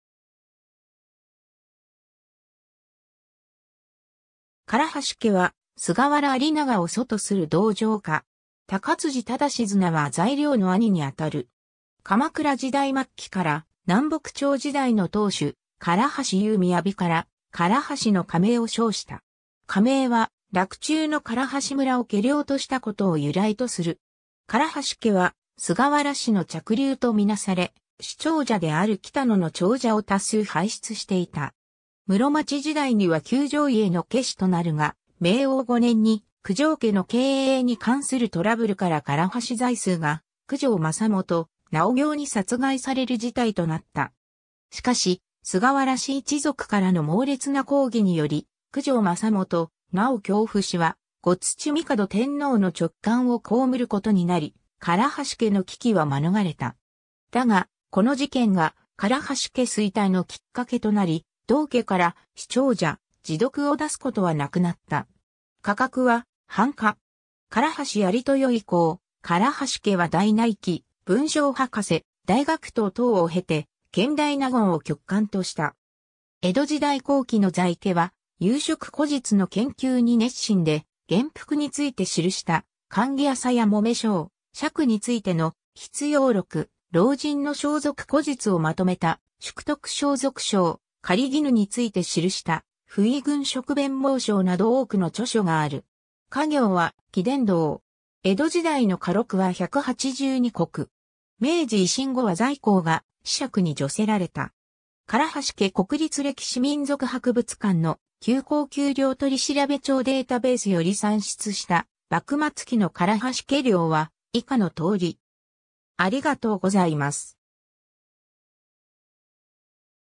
唐橋家
唐橋家（からはしけ）は菅原在良（従四位上・式部大輔・贈従三位）（1041年 - 1121年）を祖とする堂上家。
高辻是綱（正四位下・大学頭）（1030年 - 1107年）は在良の兄に当たる。
 鎌倉時代末期から南北朝時代の当主唐橋有雅（従二位・大膳大夫）（1275年 - 1356年）から唐橋の家名を称した。
 家名は、洛中の唐橋村を家領としたことを由来とする。
 唐橋家は菅原氏の嫡流とみなされ、氏長者である北野の長者を多数輩出していた。
 室町時代には九条家の家司となるが、明応5年（1496年）に九条家の経営に関するトラブルから唐橋在数（正四位下・大学頭大内記）（1448年 - 1496年）が九条政基・尚経に殺害される事態となった。
しかし、菅原氏一族からの猛烈な抗議により、九条政基・尚経父子は後土御門天皇の勅勘を蒙ることになり、唐橋家の危機は免れた。
だが、この事件が唐橋家衰退のきっかけとなり、同家から氏長者・侍読を出すことはなくなった。
 家格は半家。
唐橋在豊（1391年 - 1464年）以降、唐橋家は大内記・文章博士・大学頭等を経て、権大納言を極官とした。
 江戸時代後期の在家は有職故実の研究に熱心で、元服について記した『冠儀浅寡抄』、笏についての『畢用録』、老人の装束故実をまとめた『宿徳装束抄』、狩衣について記した『布衣群色便蒙抄』など多くの著書がある。
 家業は紀伝道。
江戸時代の家禄は182石。
明治維新後は在綱が子爵に叙せられた。
 唐橋家 国立歴史民俗博物館の『旧高旧領取調帳データベース』より算出した幕末期の唐橋家領は以下の通り。
（3村・182石5斗）